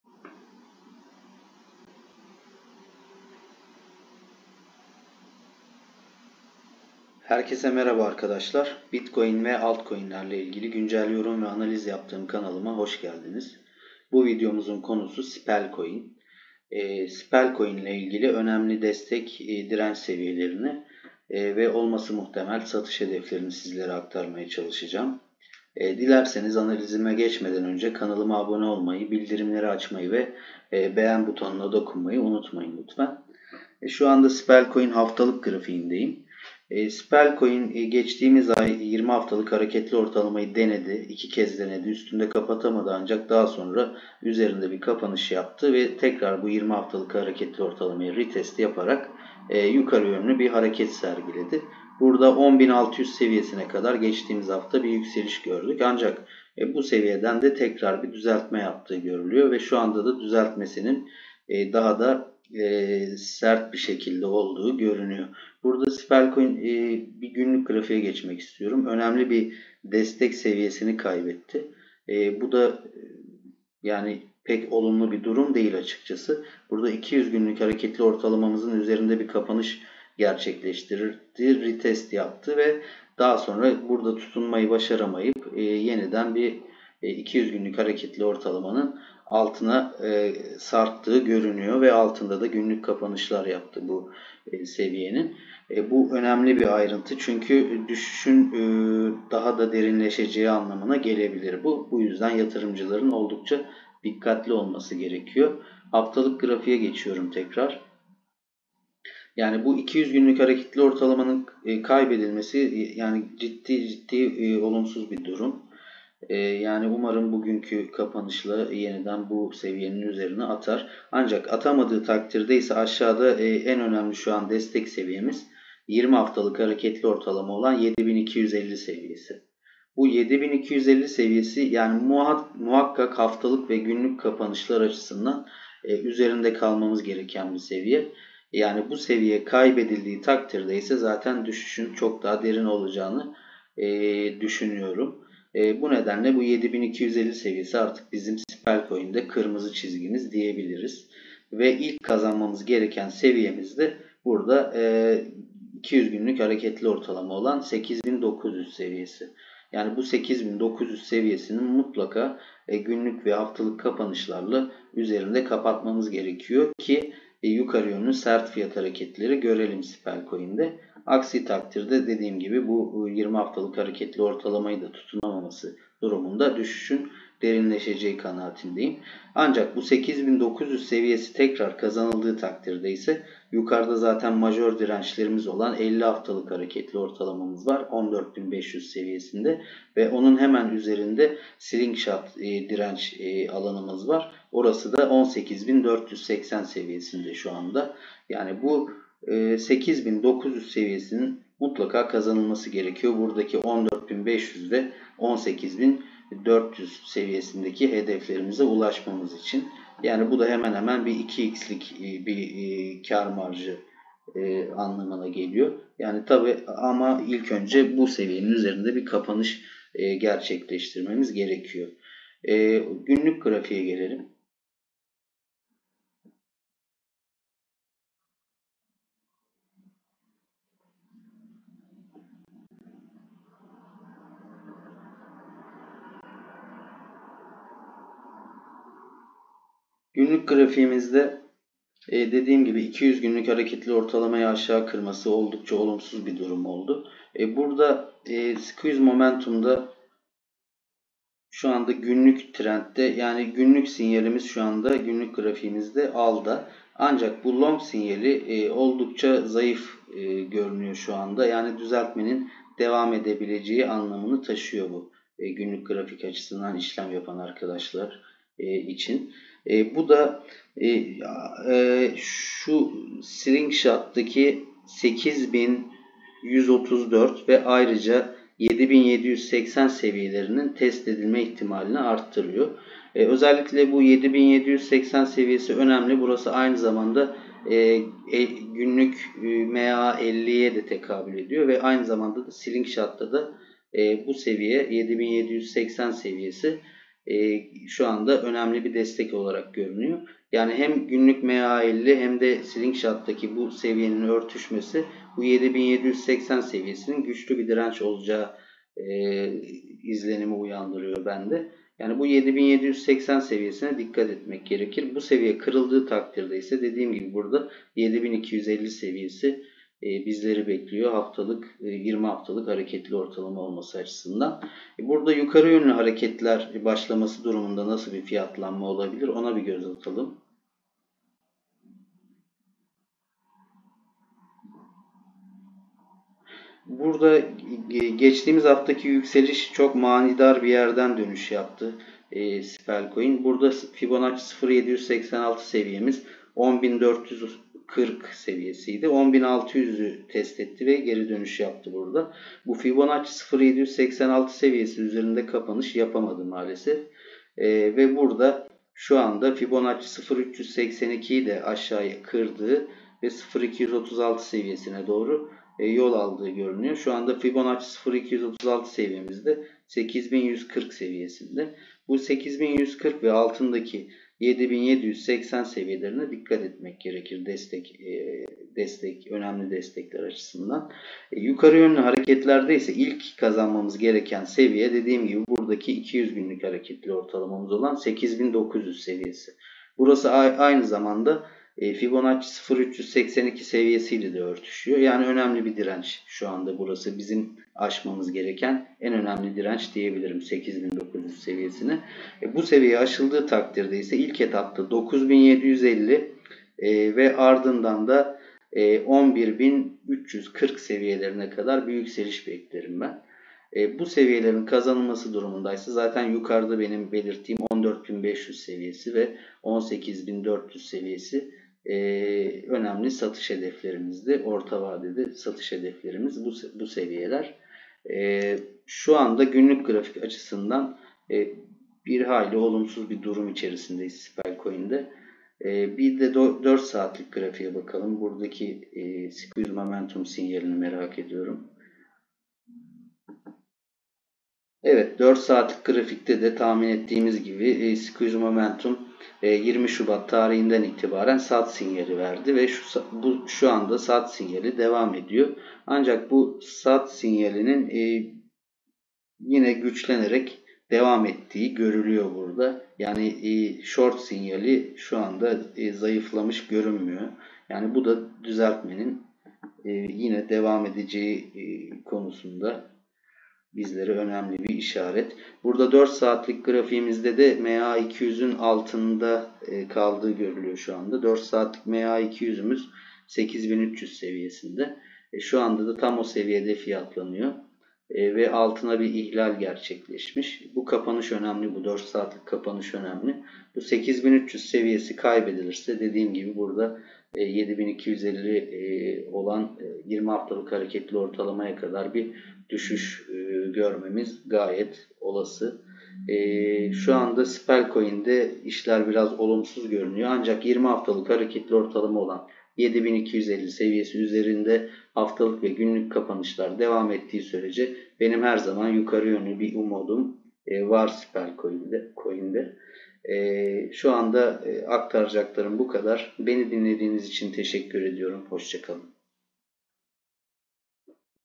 Dikkat! Spell Coin Kritik Desteği Kaybetti! Spell Coin Analiz Son Dakika Kripto Para Yorumu
Kripto para ve altcoin yatırımcıları için spell coin analiz son dakika durum ve yorumunu paylaştığım içeriğimde spell token önemli seviyeleri inceleyerek aktardım.